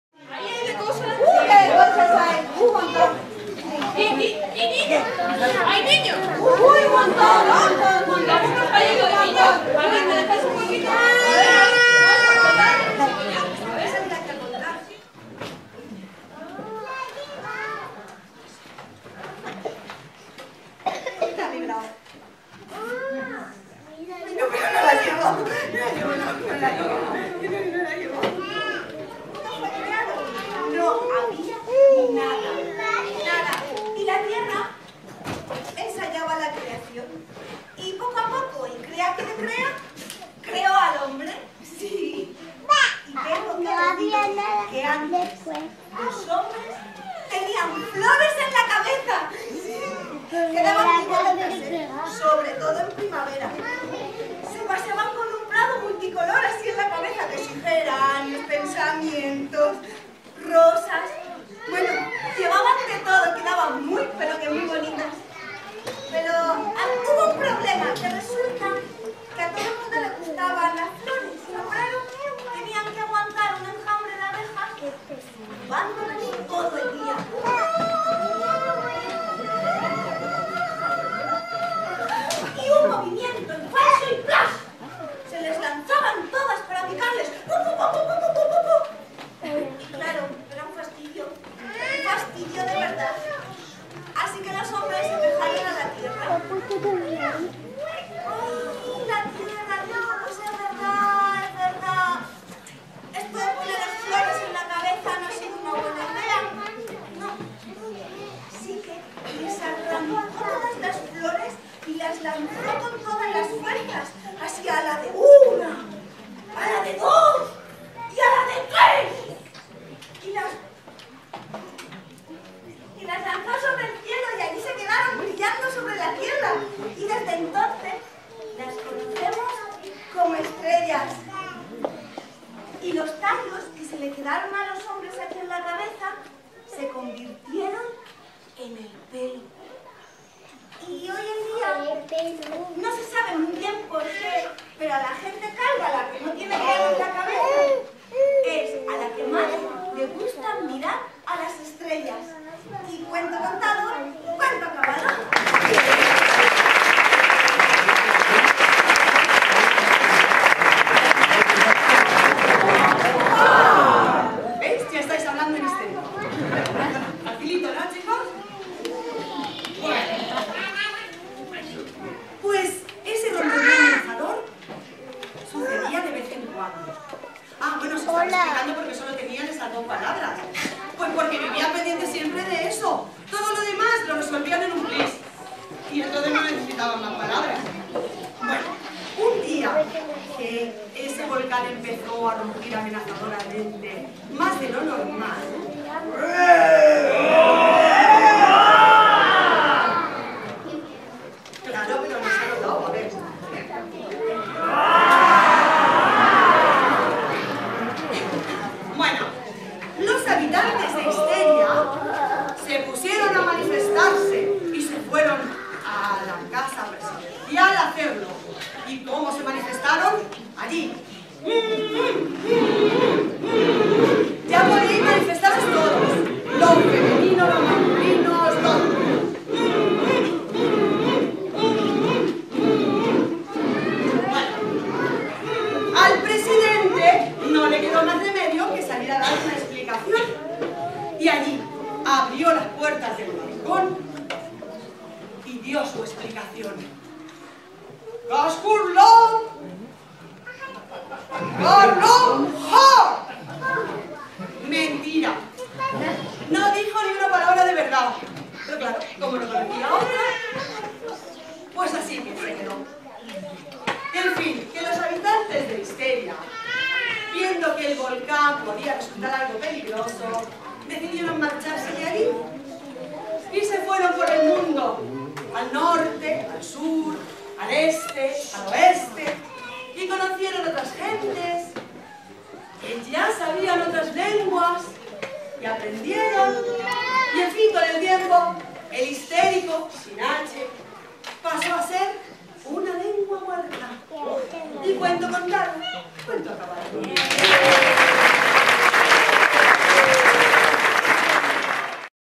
"Cuentos contados", de Inviento en la Biblioteca de Ciudad Real
La Biblioteca Pública del Estado de Ciudad Real ha vivido un sábado de cuento de la mano del grupo Inviento. Numeroso público familiar se reunía al calor del espectáculo “Cuentos Contados” en una mañana desapacible que amenazaba lluvia, ideal para disfrutar de historias divertidas y emocionantes.